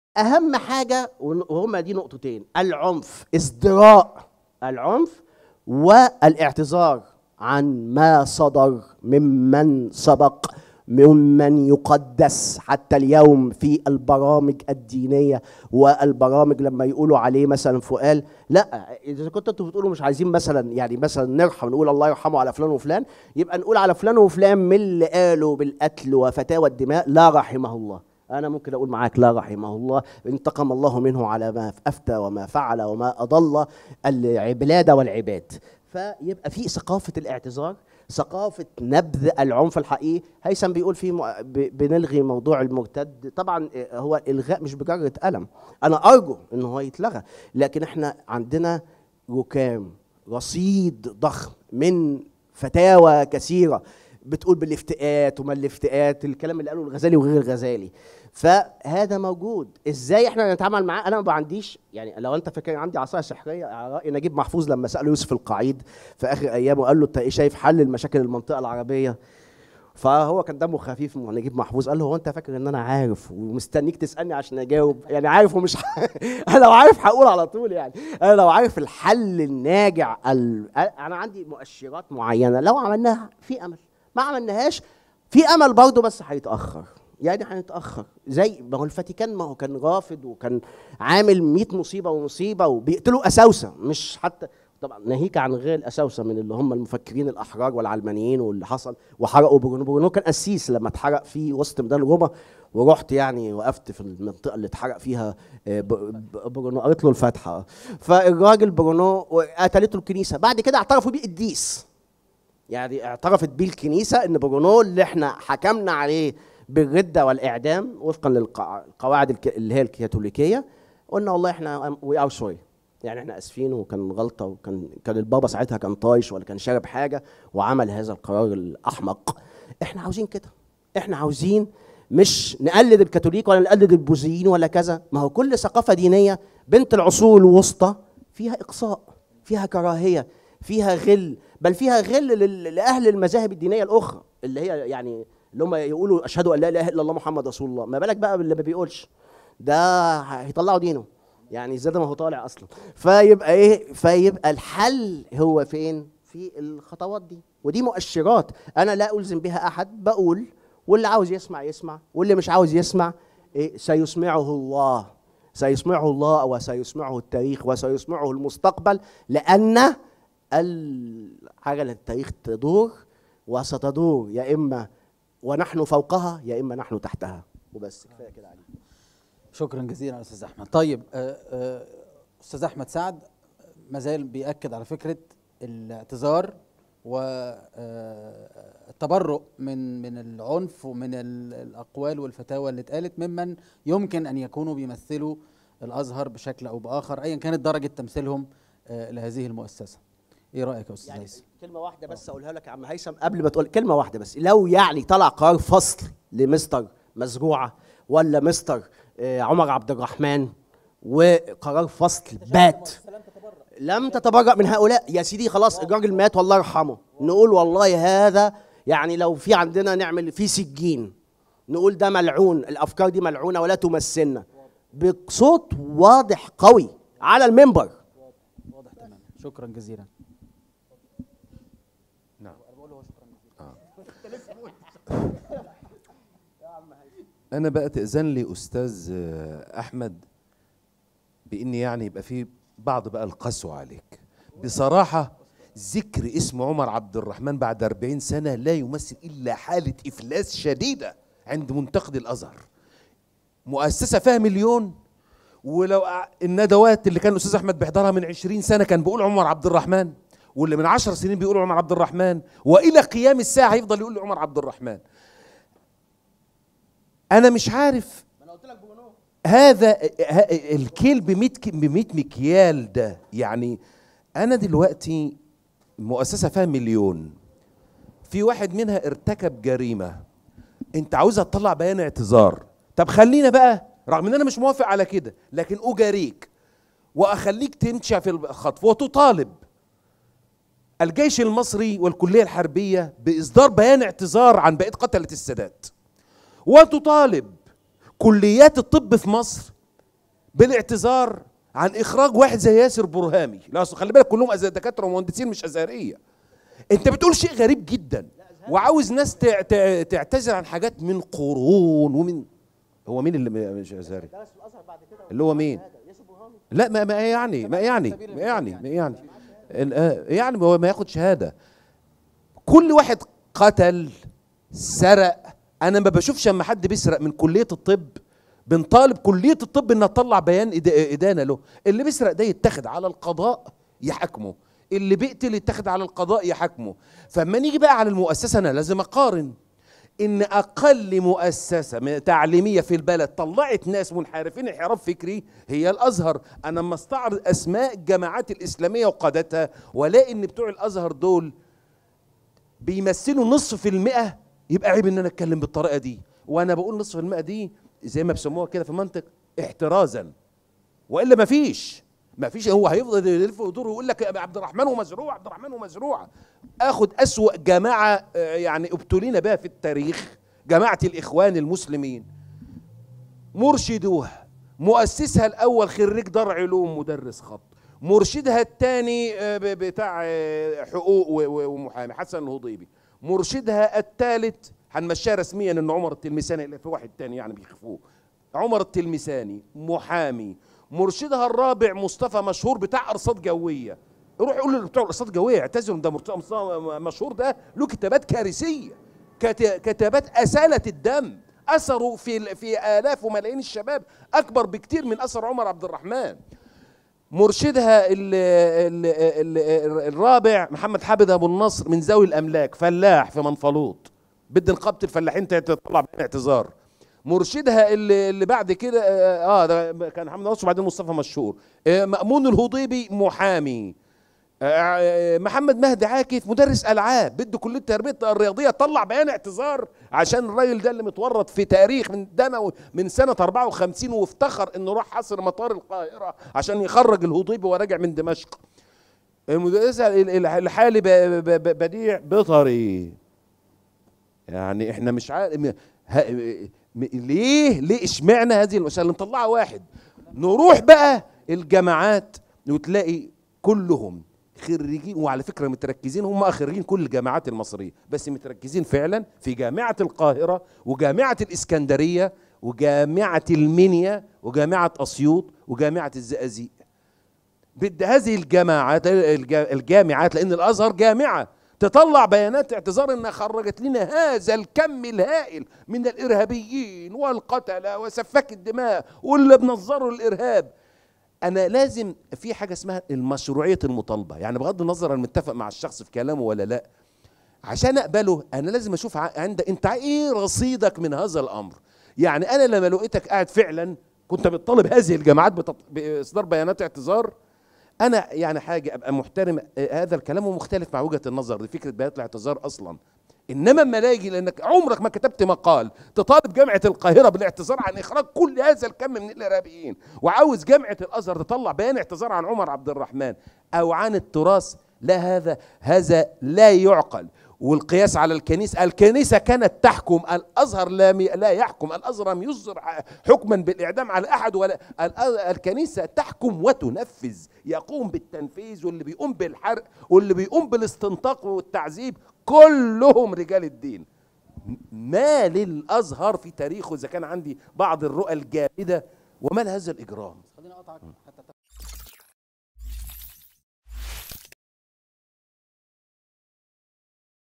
نقاش حاد بين أحمد سعد زايد وأبو زيد حول فتوى إهدار دم فرج فودة ( من ندوة لذات مصر )
سعد زايد: يجب أن يعتذر الأزهر عن فتاوى الدماء التي صدرت من بعض مشايخه القدامى.
أبو زيد: لو طالبنا الأزهر باعتذار عن فتاوى بعض منتسبيه، لطلبنا من جامعة القاهرة بالاعتذار عن جرائم ارتكبها رموز عنف انتسبوا لها يوما.
تكوين
تنوير